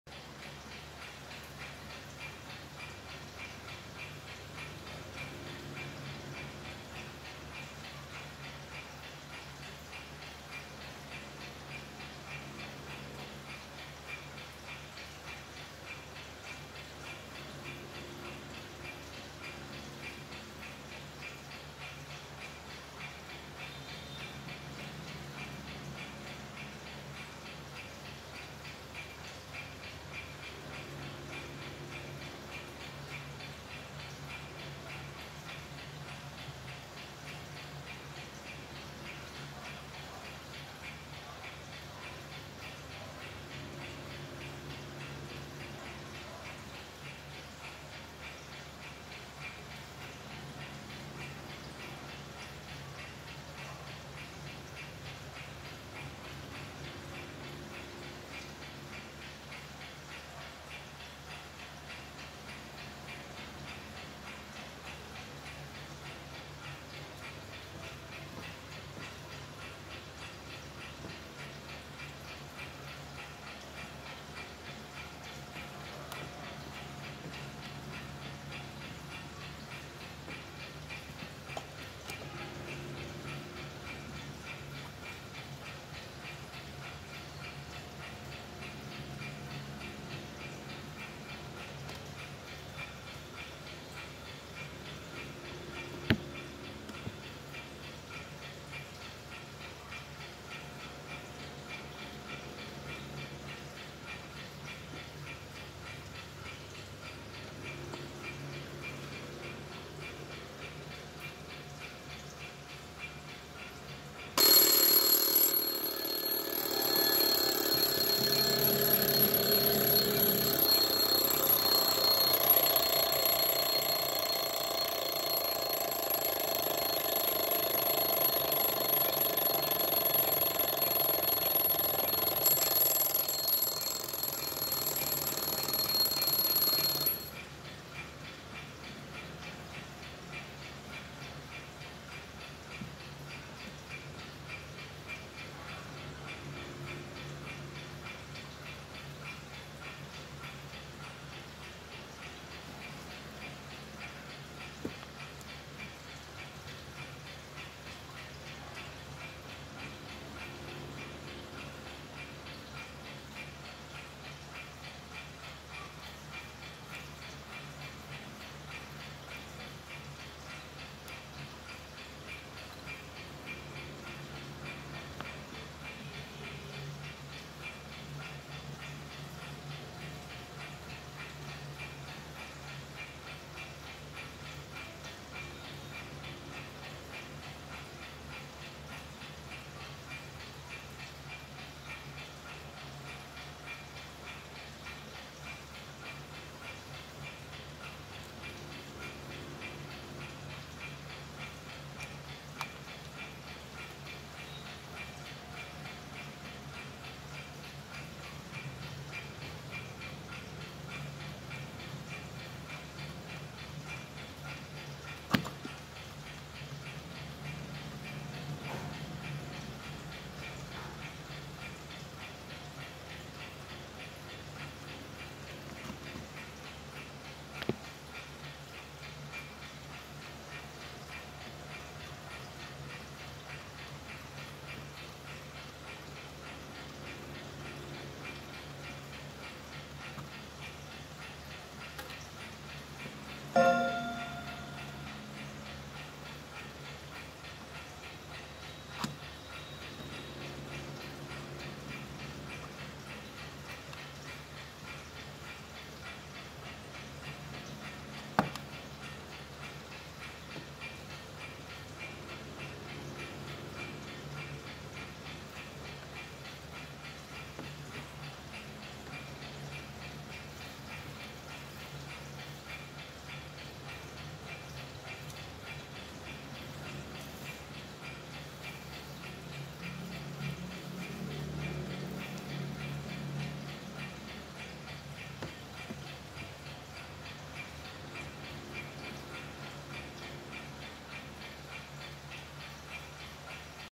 (đã bán)đồng hồ cơ báo thức có lịch hiệu hero máy 7903 toàn đồng nguyên bản
Đồng hồ xưa máy móc toàn đồng sử dụng cực kỳ bền bỉ theo thời gian cầm rất nặng tay.

Tiếng tik tak nghe khỏe đều đặn chuông to thanh giờ chuẩn

Lịch nhảy chuẩn 24h đá lịch 1 lần

Bướm núm cót vặn đầy đủ zin theo máy

Rất đáng sưu tầm hay trưng bày.